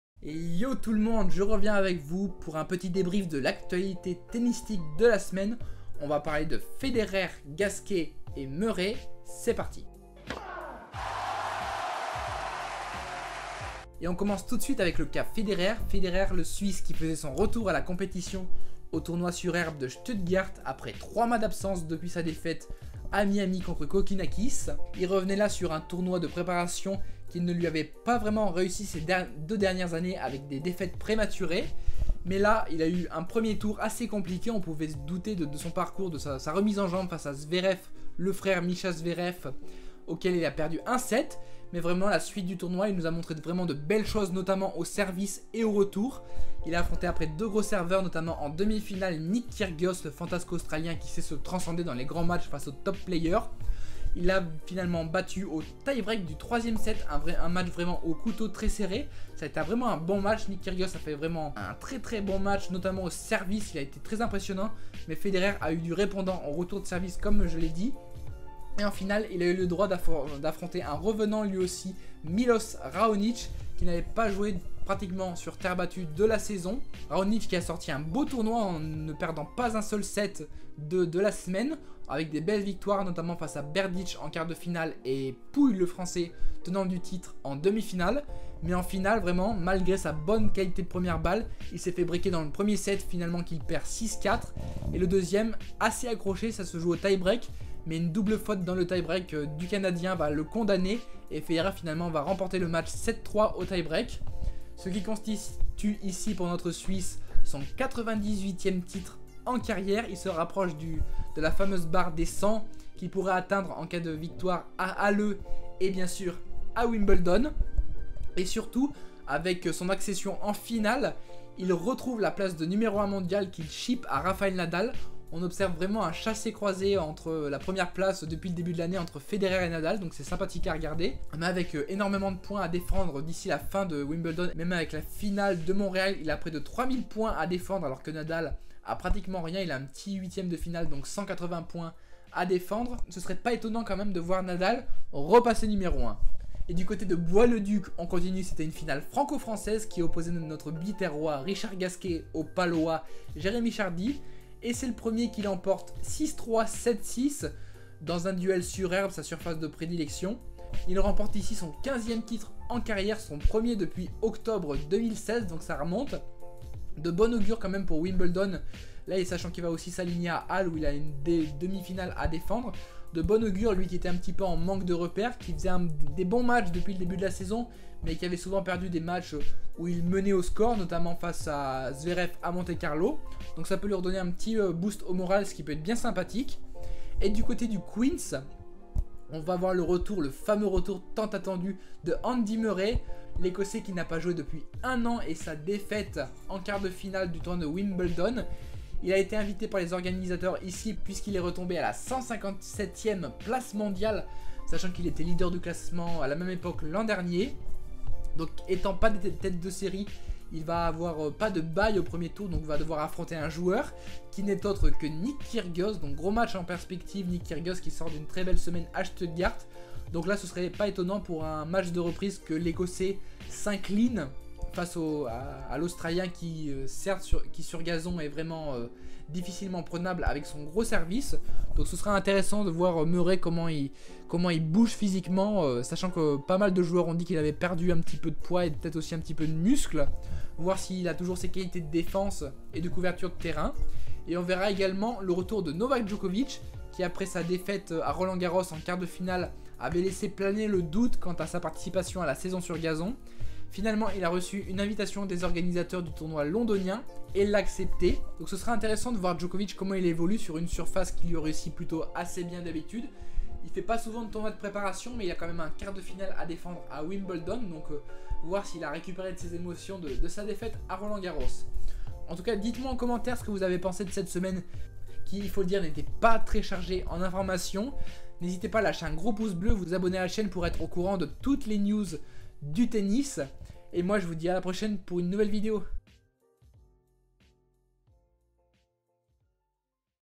LE RETOUR DE FEDERER ET MURRAY | L'actu de la semaine
Le gazon est enfin de retour ! Au programme cette semaine, les tournois de Stuttgart et de S'Hertogenbosch où Federer et Gasquet ont brillé. Le suisse retrouve par ailleurs la 1ere place mondiale.
Benoit vous donne également des nouvelles d'Andy Murray, disparu des courts depuis 1 an qui fera son grand retour au Queens la semaine prochaine.